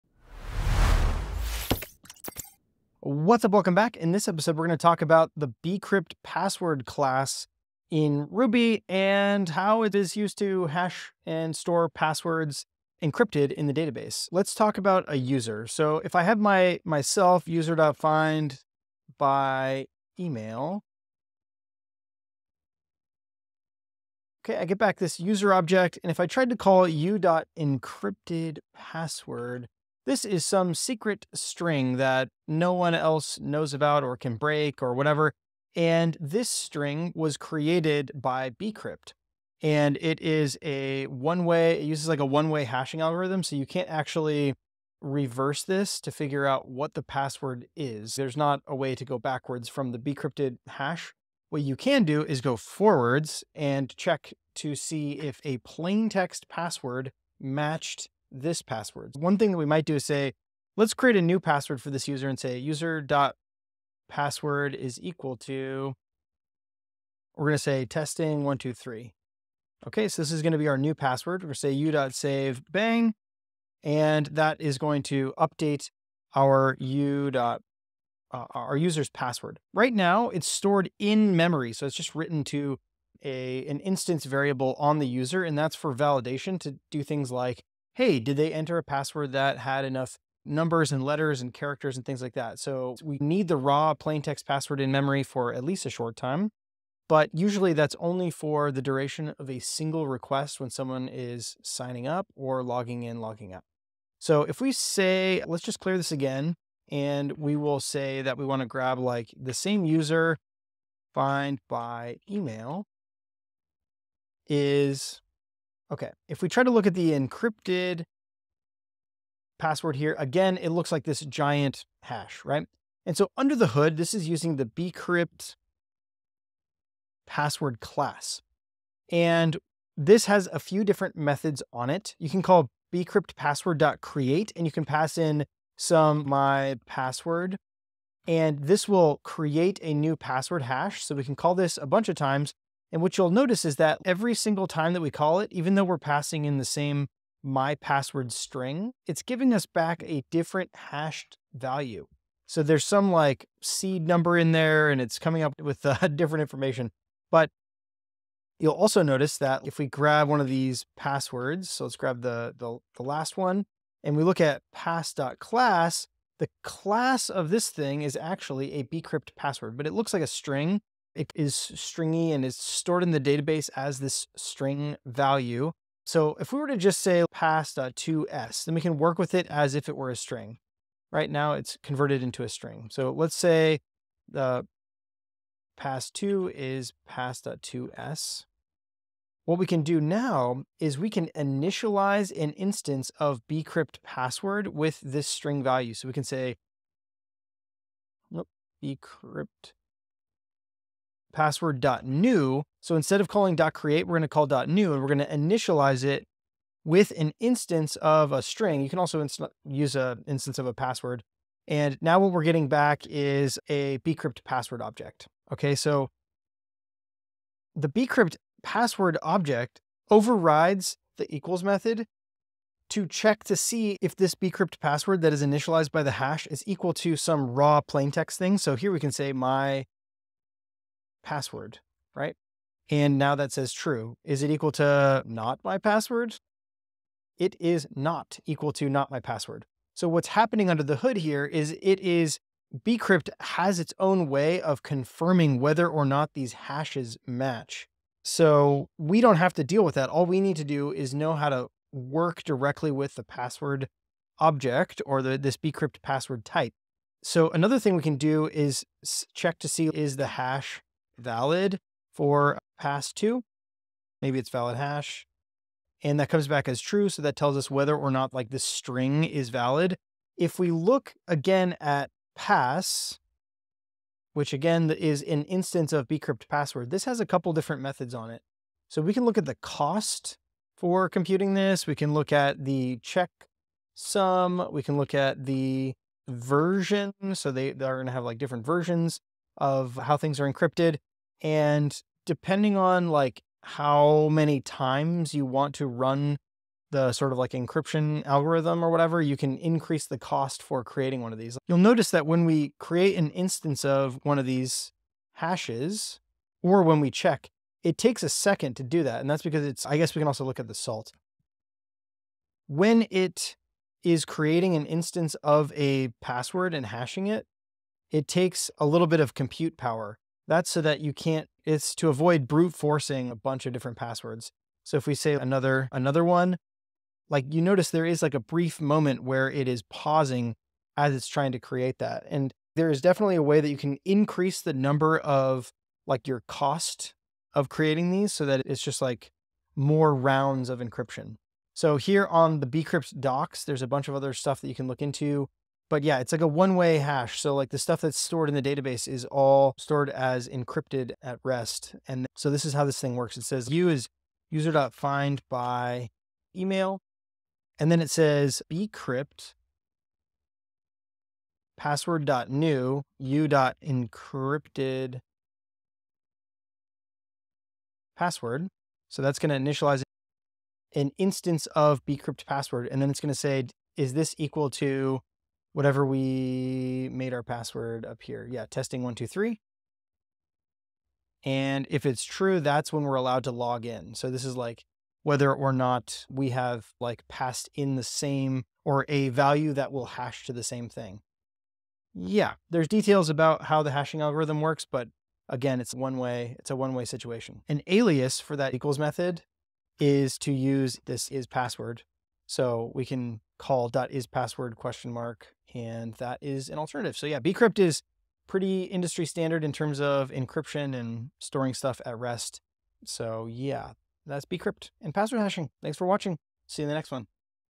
BCrypt Password basics in Ruby
This video explains how to use the Bcrypt password class in Ruby to securely hash and store passwords. We'll look at how a user's encrypted password is stored in the database as a hashed string that can't be reversed. To check if a password matches, you initialize a Bcrypt::Password object with the hashed password, then compare it to a plain text password using the == operator, which Bcrypt overrides to check if the hashes match.

Bcrypt handles salting and key stretching internally to make brute force attacks harder. When creating a hash, Bcrypt takes time to run its key derivation function, which can be configured by increasing the cost parameter. We'll look at how to check properties of a bcrypt password like cost, version, and valid hash.

Overall, bcrypt provides a simple interface in Ruby for securely hashing passwords. By storing the bcrypt password hashes rather than plain text passwords, the passwords are securely encrypted at rest in the database. Bcrypt is an industry-standard for password hashing.